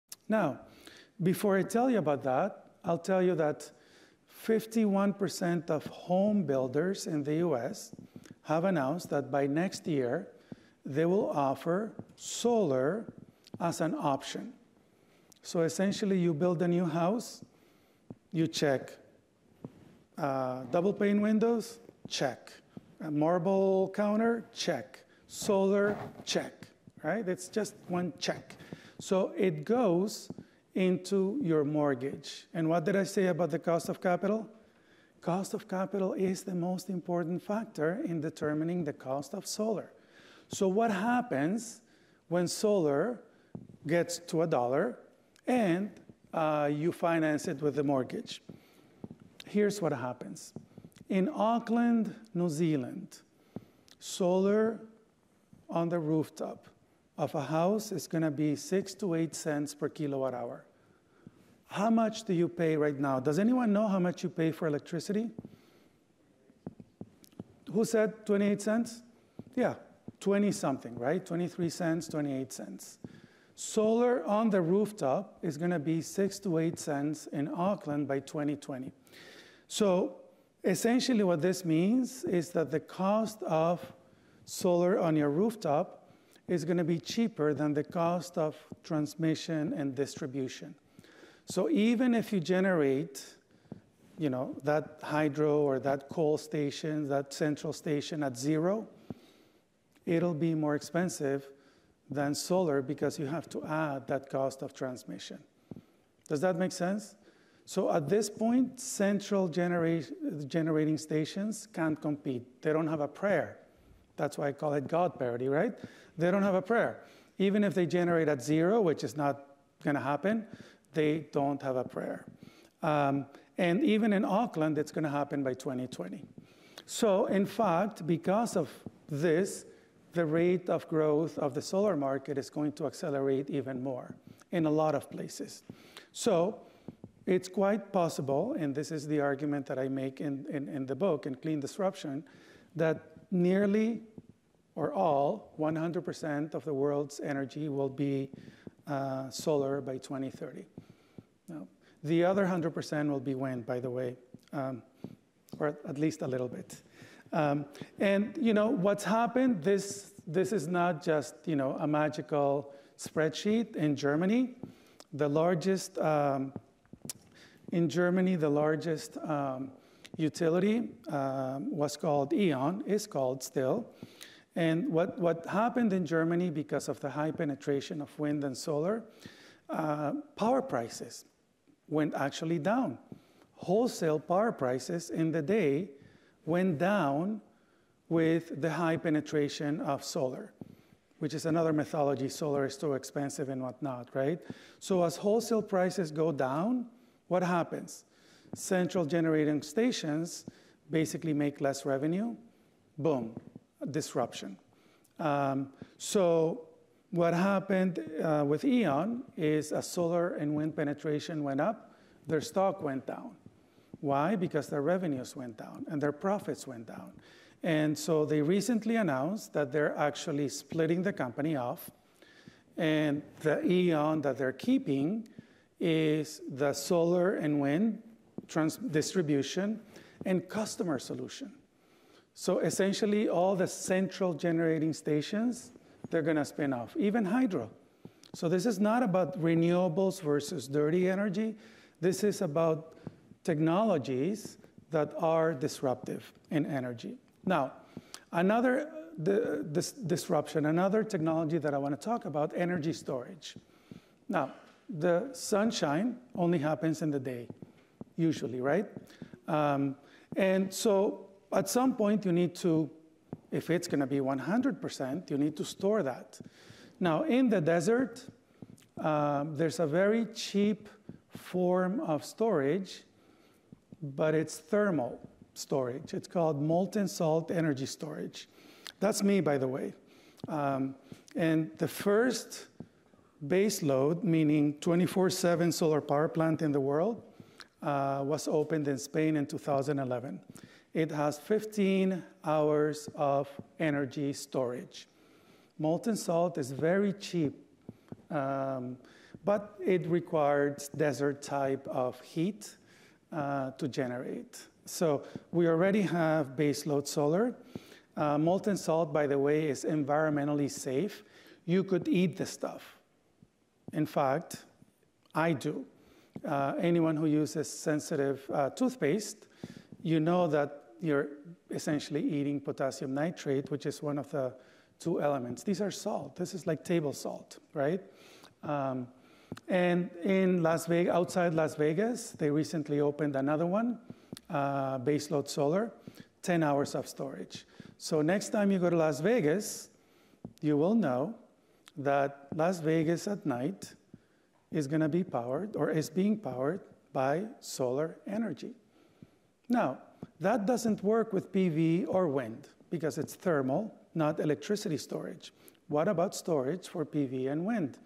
Tony Seba (4): Electric industry disruption oversas
Part 4 of Tony Seba's public lecture in Wellington, NZ on March 31 2015